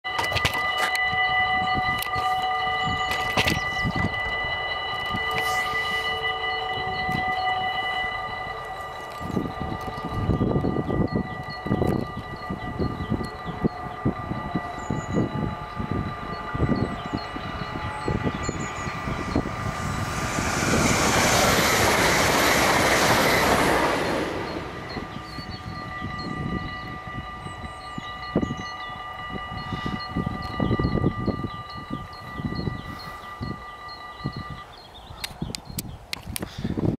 Voormalige spoorwegovergang Zwolle // Dutch Railroad Crossing
Locatie: Hessenweg
Traject: Zwolle - Meppel
Soort: AHOB
Opgeheven: 16 juli 2022
Passeren: SNG als SPR Leeuwarden - Zwolle